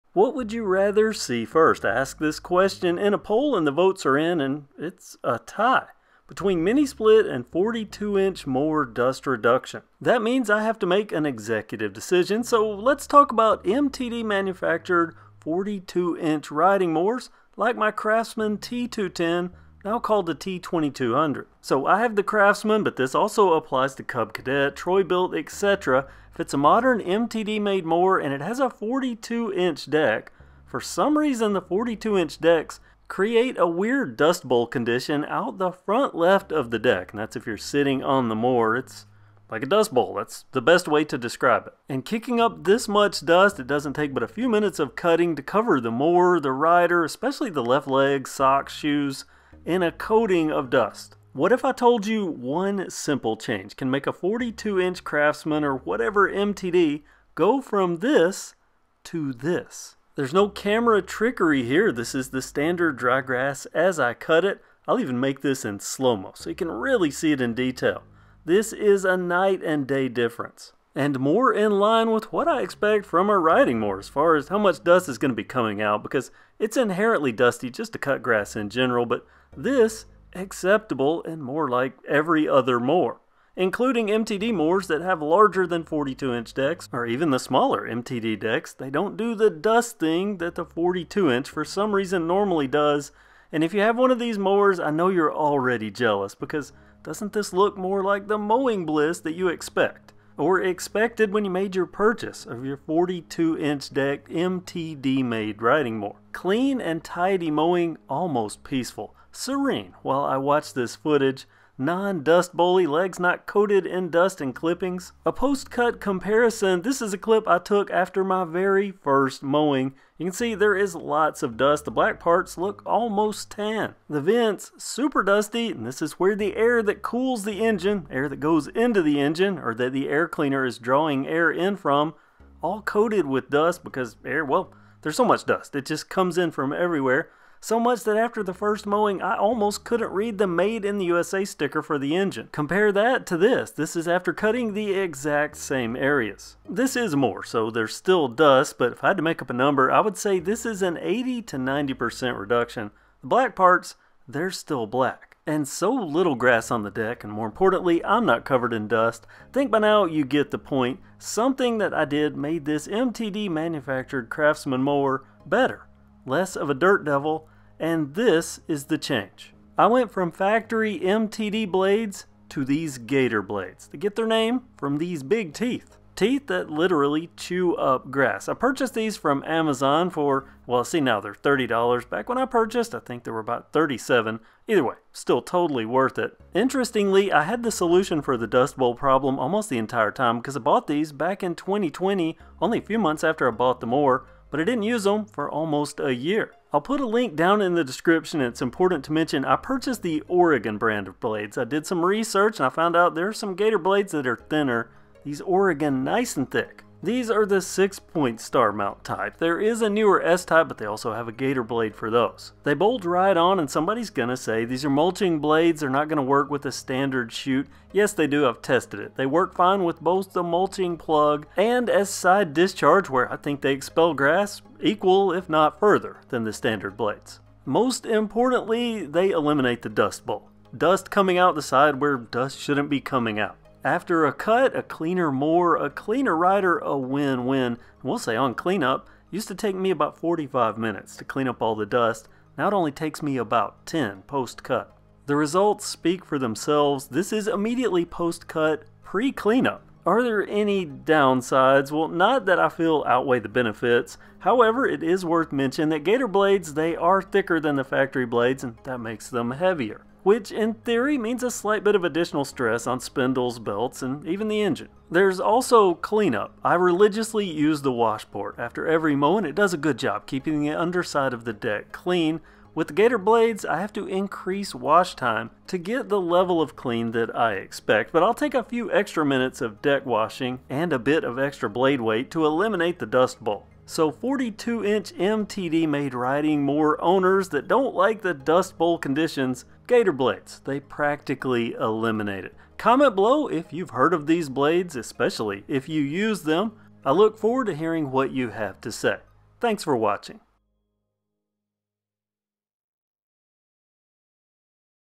DUST BOWL ELIMINATION for MTD 42-inch Riding Mowers - Craftsman T210 T2200 | Cub Cadet | Troy-bilt
Owners of Riding Mowers made by MTD (Craftsman - Cub Cadet - Troy-bilt, etc) know of the excess dust the 42" deck models create. It is an anomaly of the 42-inch models. Fortunately, there is a fix! I have discovered that Gator mulching blades practically eliminate the excess dust from the front left of the cutting deck.

Gator Blades seen in this video